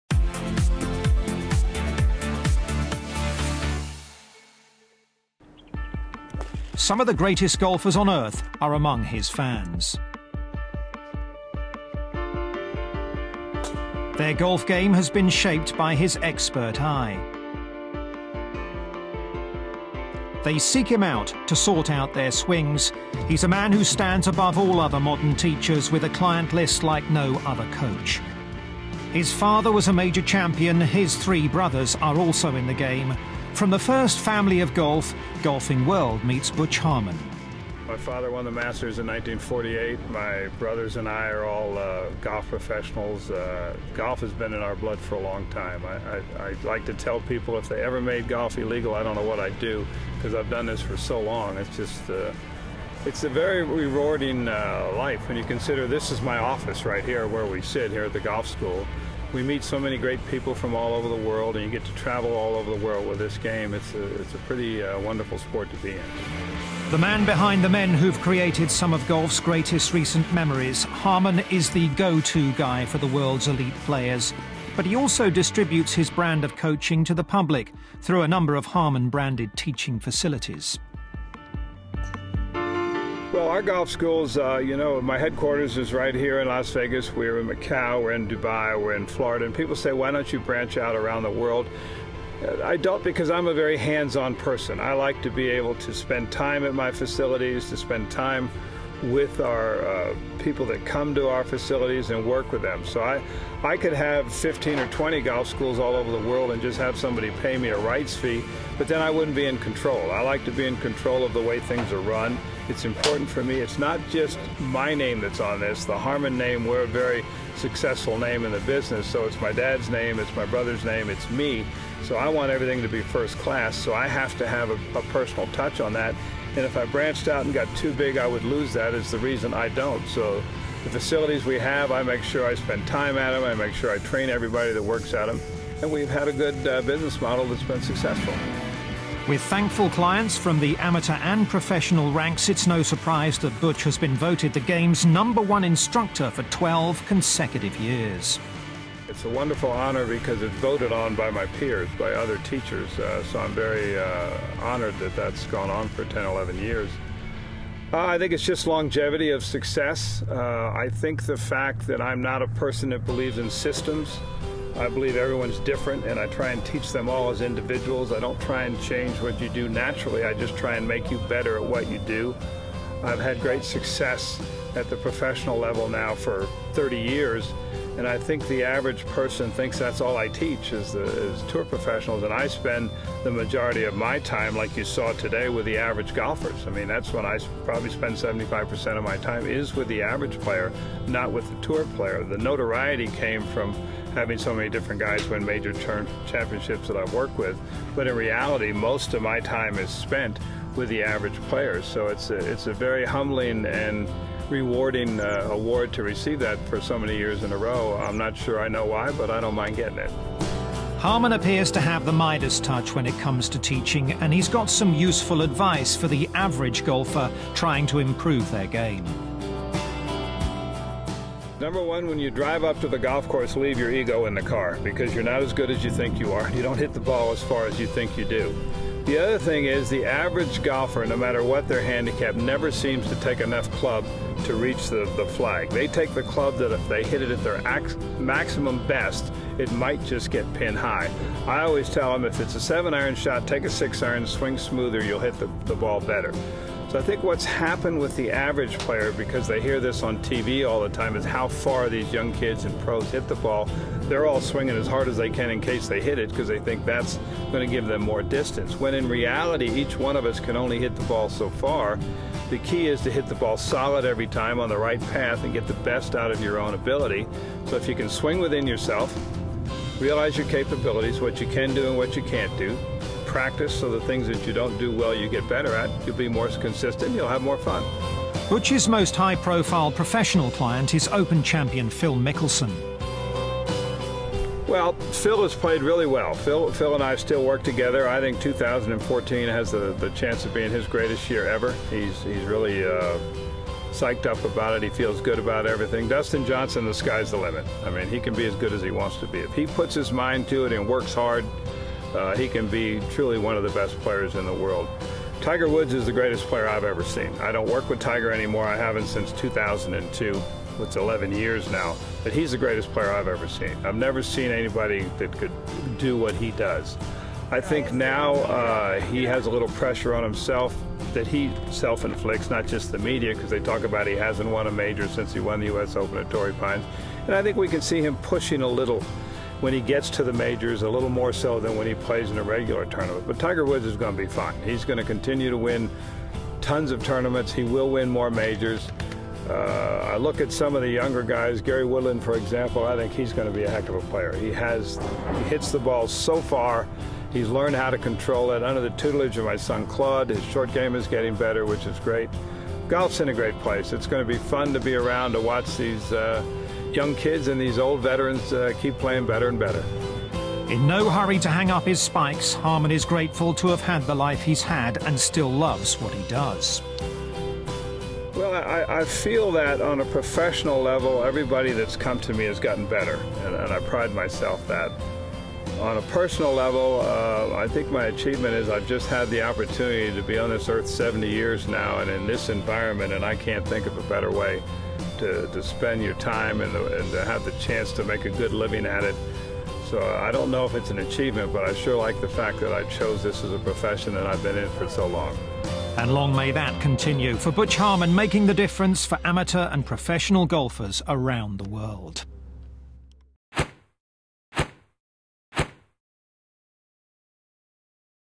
GW Player Profile: with Butch Harmon
GWTV meets Butch Harmon, one the most renowned golf coaches in the world